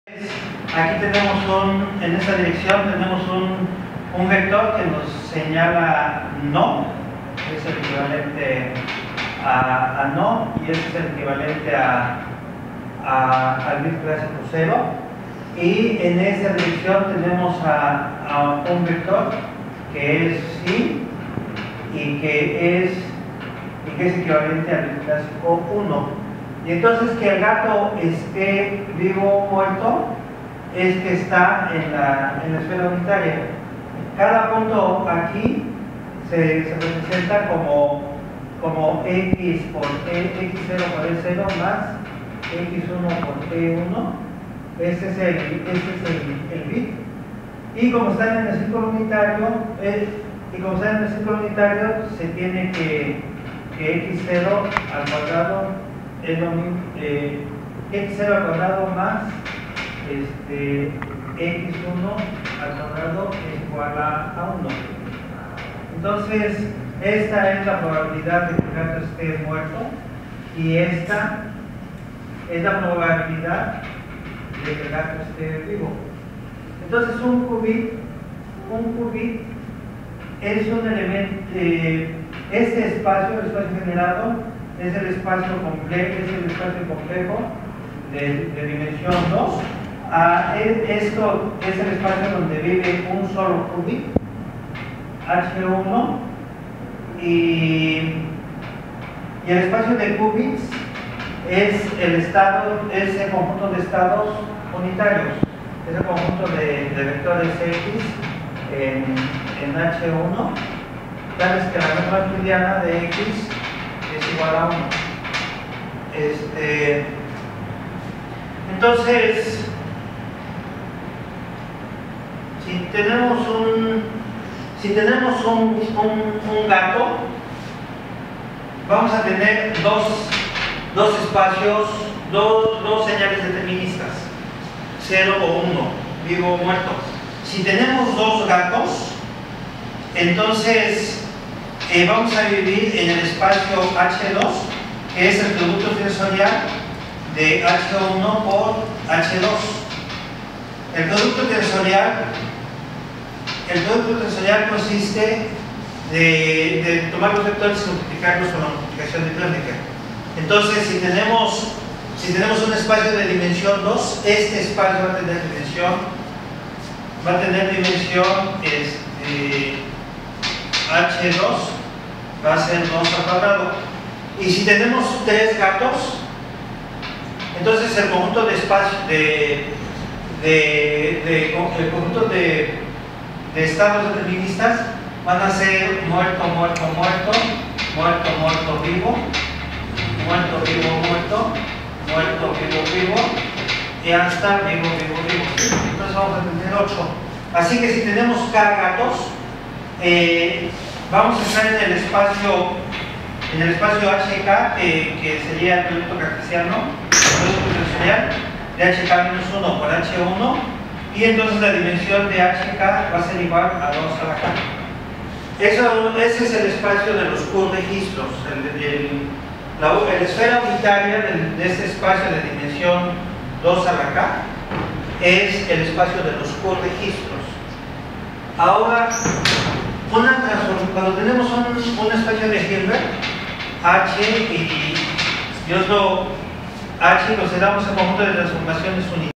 IV Encuentro Cuba-México de Métodos Numéricos y Optimización
Conferencia invitada: Solving NP-hard combinatorial optimization problems with adiabatic quantum computing. Impartida pro el professor Guillermo Morales Luna del CINVESTAV-IPN, México el jueves 22 de enero de 2015.